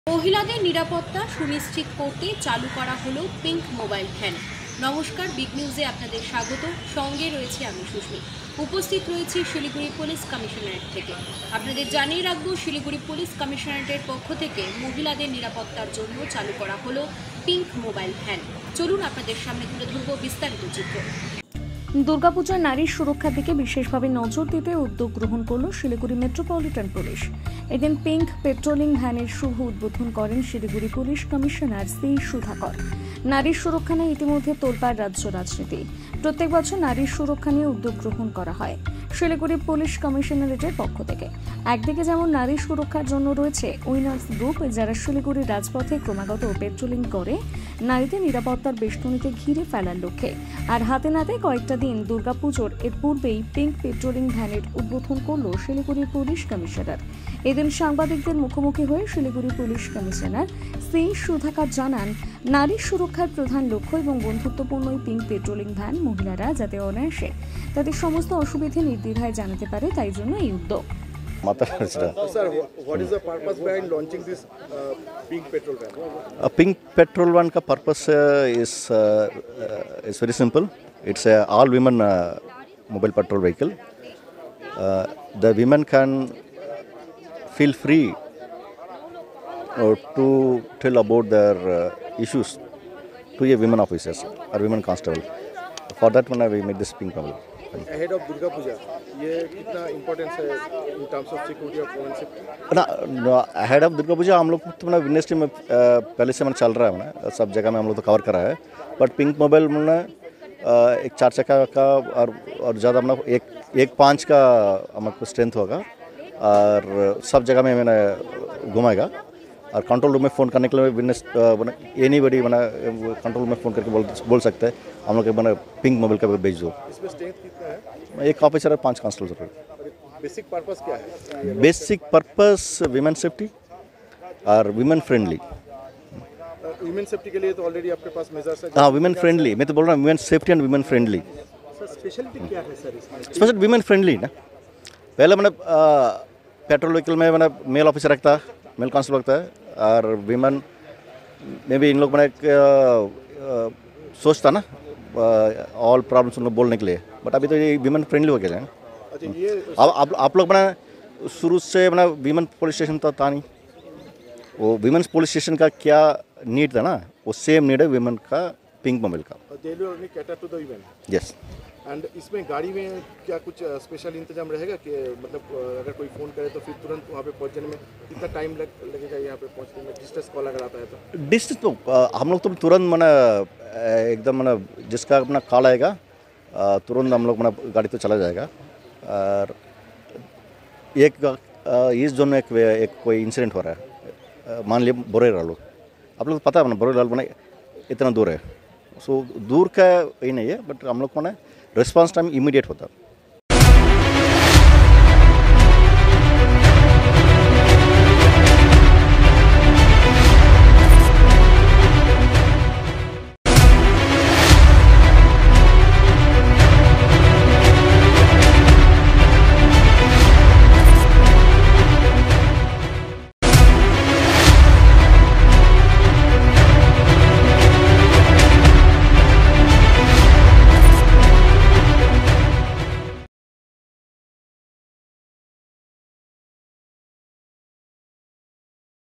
নারী সুরক্ষা সুনিশ্চিত করতে পিঙ্ক পেট্রোলিং ভ্যানের শুভ সূচনা শিলিগুড়ি পুলিশ কমিশনারেটের
শিলিগুড়ি: দুর্গাপুজোয় নারী সুরক্ষার দিকে বিশেষভাবে নজর দিতে উদ্যোগ গ্রহণ করল শিলিগুড়ি মেট্রোপলিটন পুলিশ। এদিন পিংক পেট্রলিং ভ্যানের শুভ উদ্বোধন করেন শিলিগুড়ি পুলিশ কমিশনার সি সুধাকর । নারী সুরক্ষা নিয়ে ইতিমধ্যে তোলপাড় রাজ্য রাজনীতি। প্রত্যেক বছরই নারী সুরক্ষা নিয়ে উদ্যোগ গ্রহণ করা হয় শিলিগুড়ি পুলিশ কমিশনারেটের পক্ষ থেকে। একদিকে যেমন নারী সুরক্ষার জন্য রয়েছে উইনারস গ্রুপ , যারা শিলিগুড়ির রাজপথে ক্রমাগত পেট্রোলিং করে নারীদের নিরাপত্তার বেষ্টনীতে ঘিরে ফেলার লক্ষ্যে । আর হাতেগোনা কয়েকটা দিন দুর্গাপুজোর এর পূর্বেই পিঙ্ক পেট্রলিং ভ্যান এর উদ্বোধন করল শিলিগুড়ি পুলিশ কমিশনার। এদিন সাংবাদিকদের মুখোমুখি হয়ে শিলিগুড়ি পুলিশ কমিশনার সি সুধাকর জানান, নারী সুরক্ষা এর প্রধান লক্ষ্য, এবং বন্ধুত্বপূর্ণ এই পিংক পেট্রোলিং ভ্যান, মহিলারা যাতে অনায়াসে তাদের সমস্ত অসুবিধা নির্দ্বিধায় জানাতে পারে তাই এই উদ্যোগ।